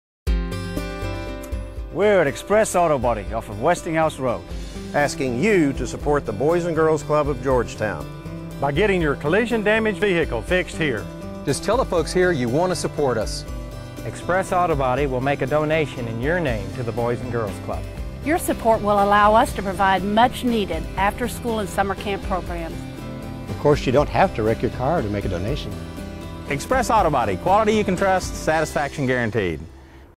Express Autobody and the Boys and Girls Club of Georgetown commercial
Express Autobody of Georgetown Texas has teamed up with the Boys and Girls Club of Georgetown to raise money to provide much needed after school and summer camp programs for the youth in our community.  If this ad is mentioned when your vehicle is dropped off for repairs we will make a donation of up to $100 in your name to the Boys and Girls Club of Georgetown.